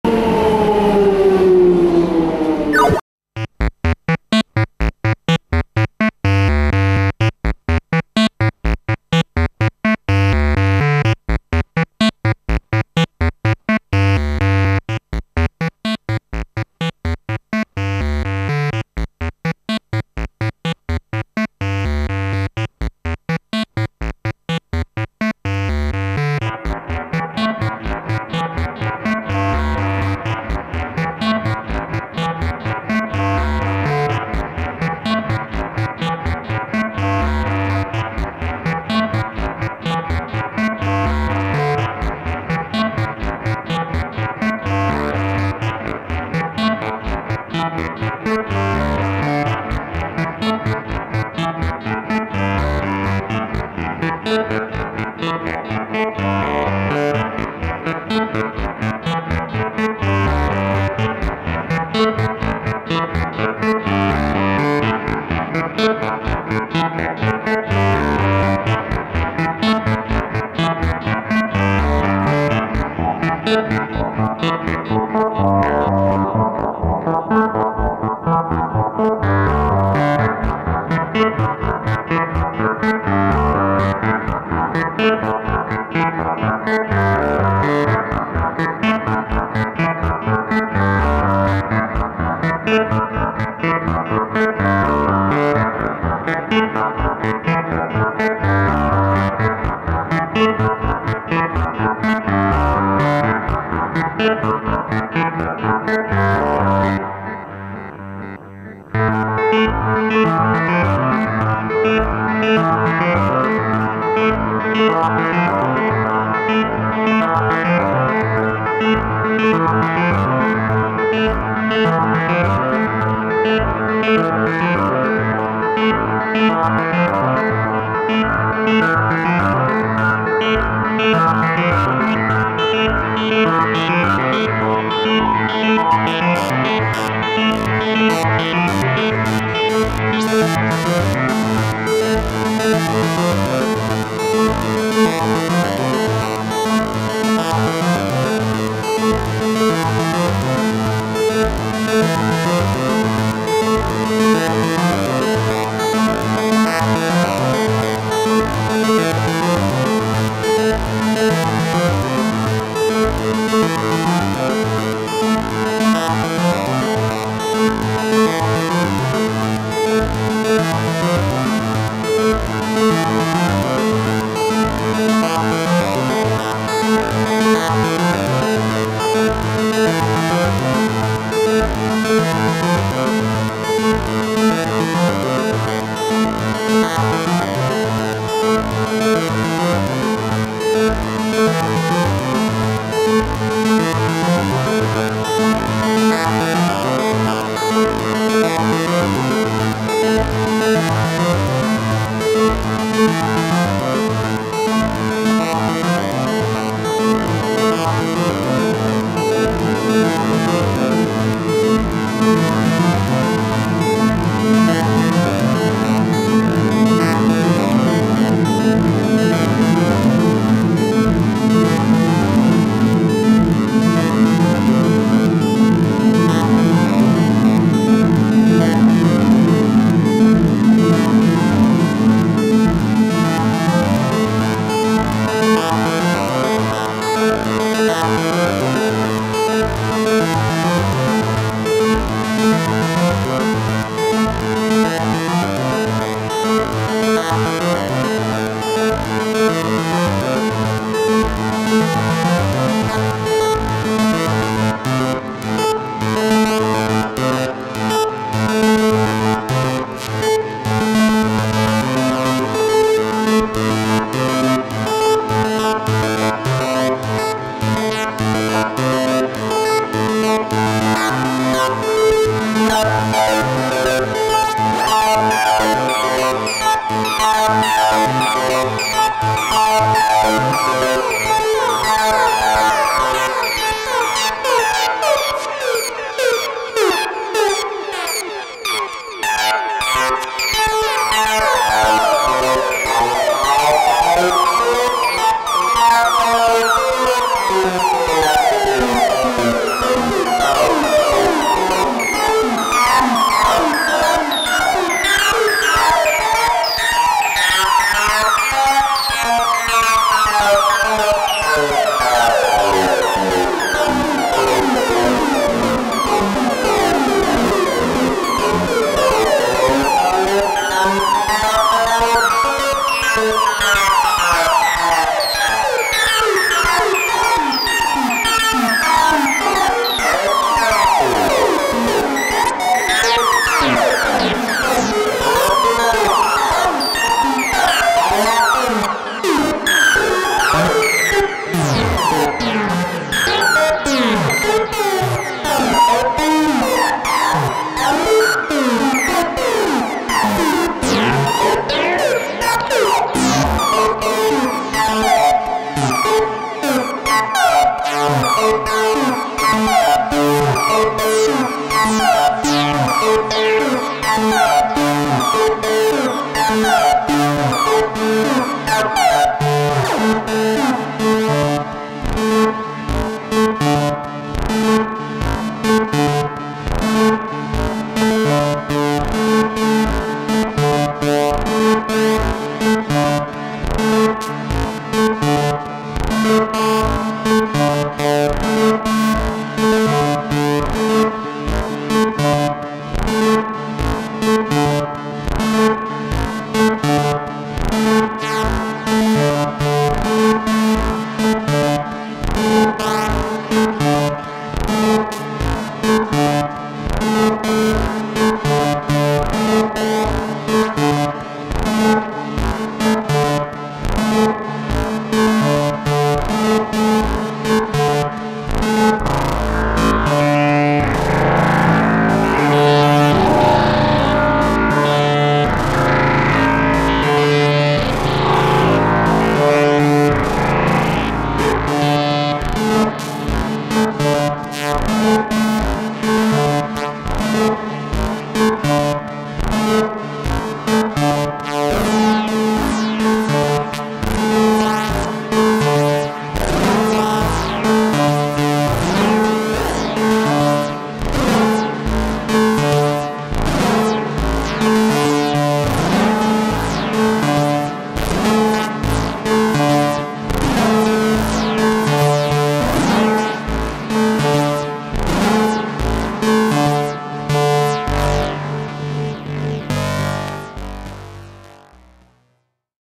Vermona Perfourmer MKII Four Voice Analog Synthesizer.
Vermona Perfourmer MK II Four Voice Analog Synthesizer, fed through a Roland RE-20 Space Echo.Then two Behringer MIC200 Ultragains (Audiophile Vacuum Tube Preamplifier with Preamp Modeling Technology) set to Keyb.